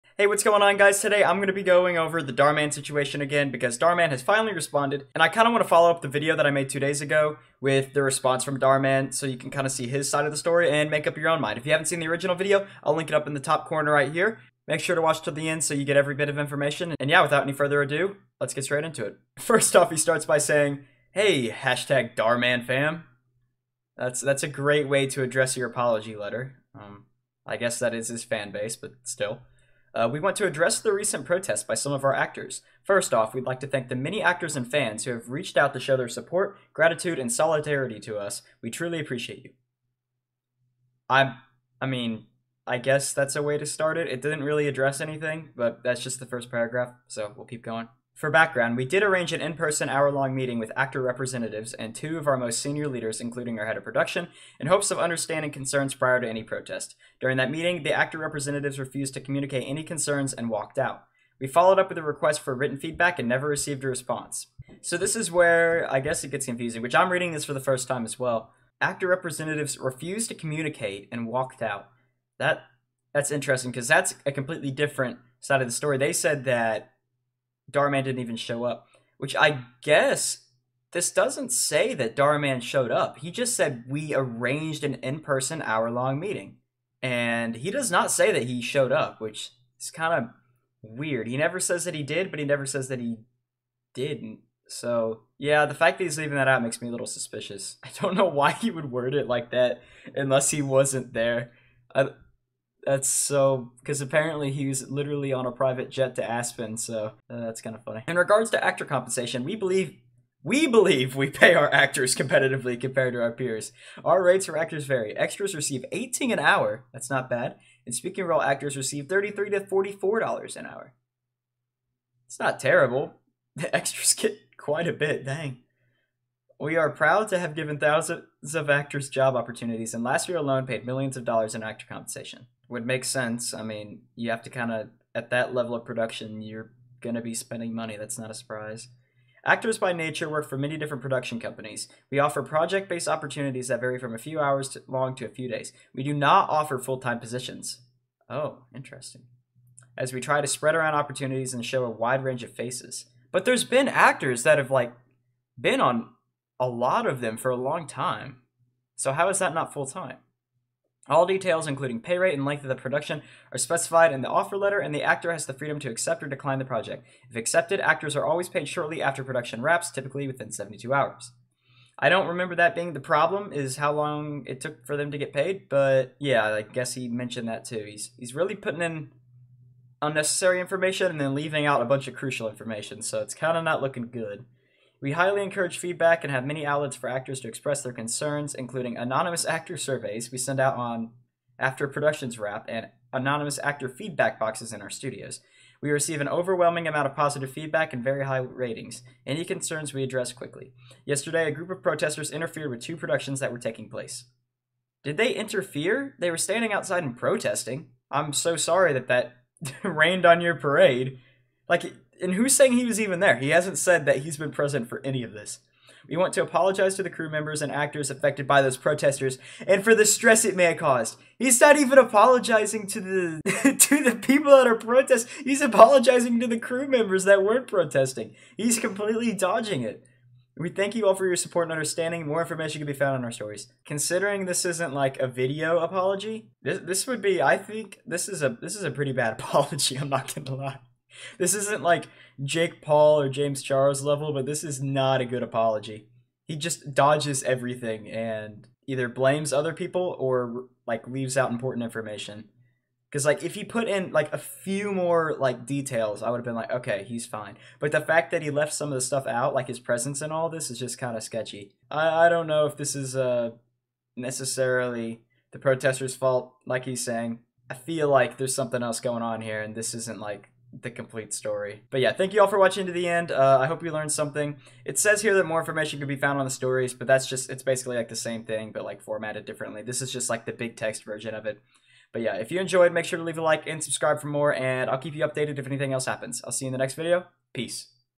Dhar Mann Responds to His Protest Drama... (apology statement)
Today I cover the recent apology Dhar Mann has sent out that adresses the recent protest drama and controversy he faces from his actors. This is a pretty bad apology in my opinion and he could've done a little better at verifying some important information. Dhar Mann studios is currently in trouble but hopefully this tiktok and twitter drama can get sorted out well as well as soon.